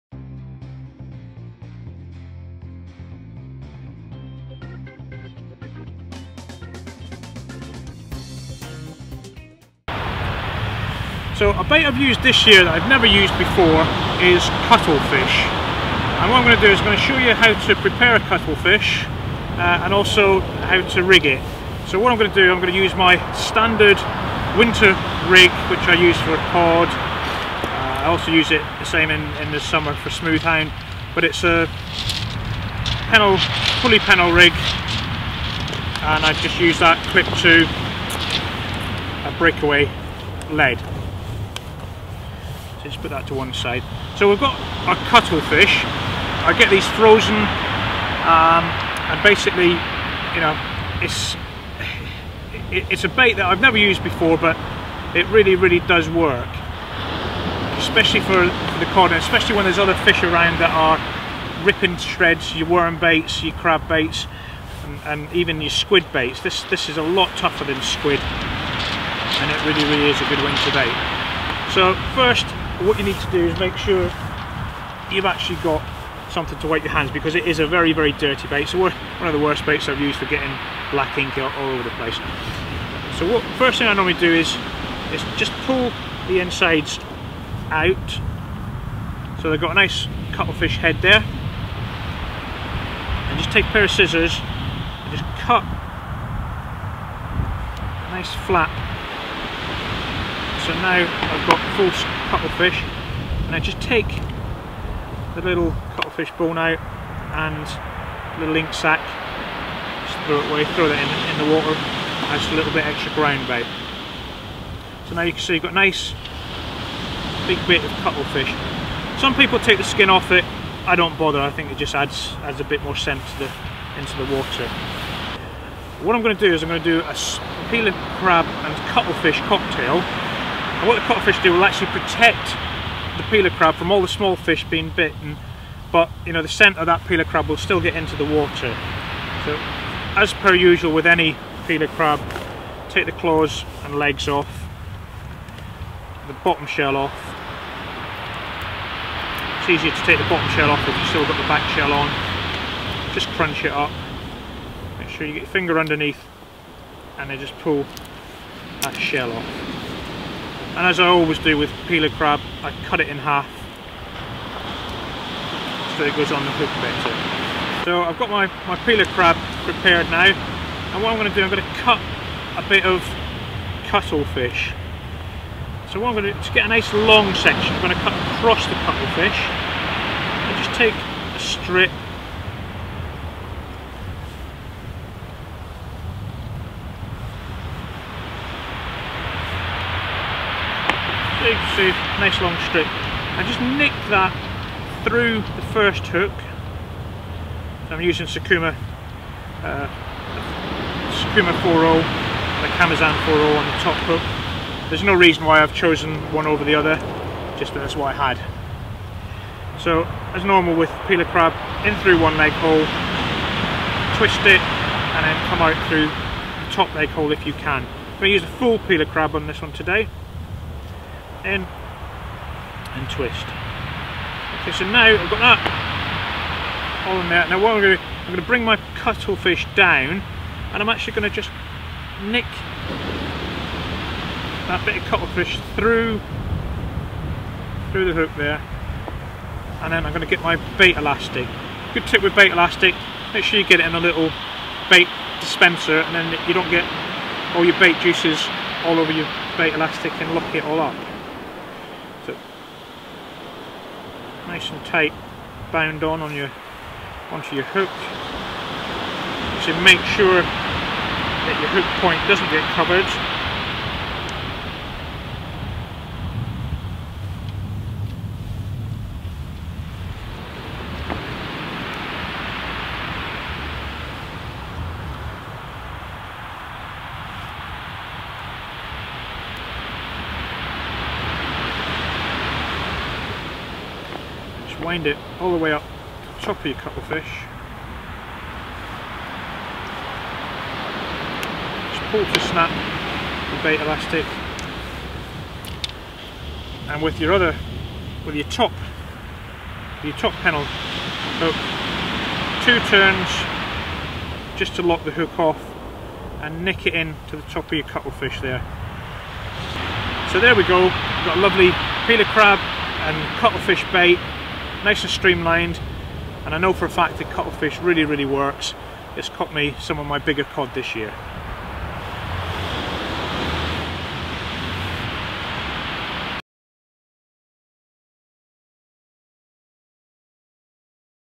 Whats the best bait for Cod : How to rig cuttlefish and Peeler Crab cocktail
Cuttlefish and Peeler crab are arguably the best baits for winter Cod, but combine the two together and you have a killer bait. This video shows the best way to rig this fish cathing cocktail.